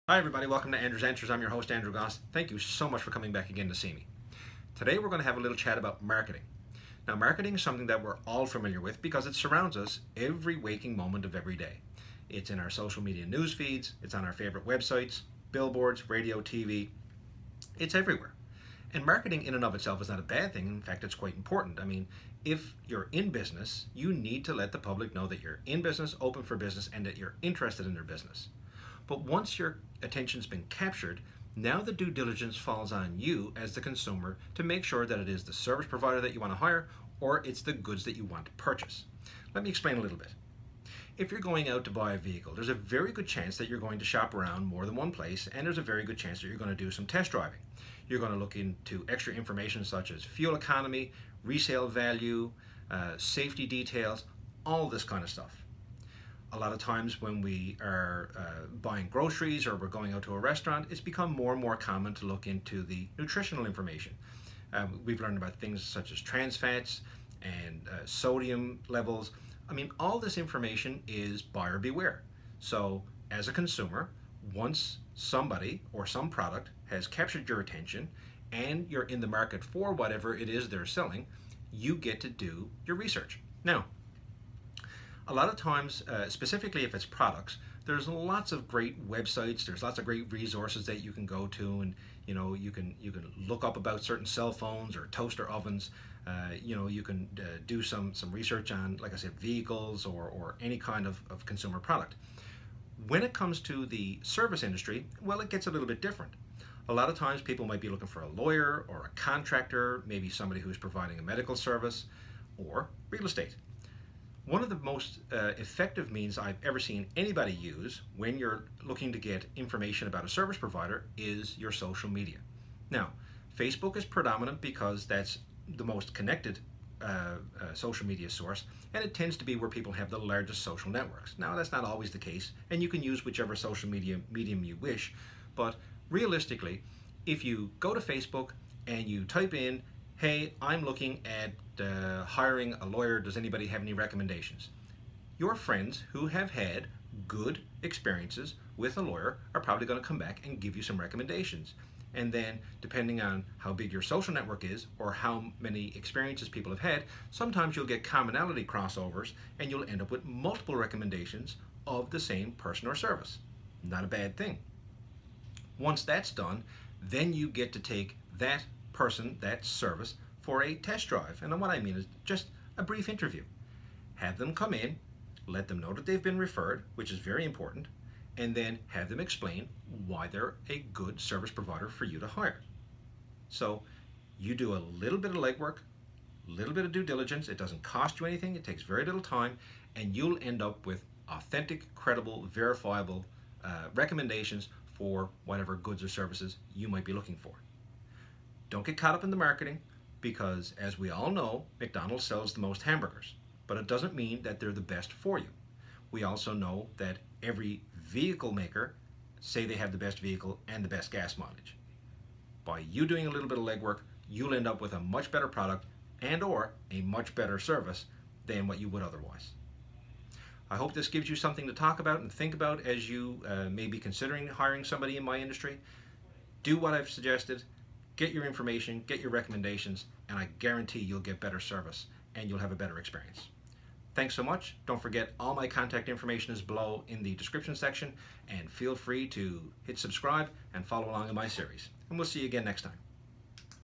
Ep. 14 A Consumers Guide to Marketing
Marketing is designed to capture your attention however it is the consumers responsibility to fact check the marketing and decide if what they've seen is what they want.

Text/Call Me Anytime

"I am never too busy for you and always appreciate your referrals"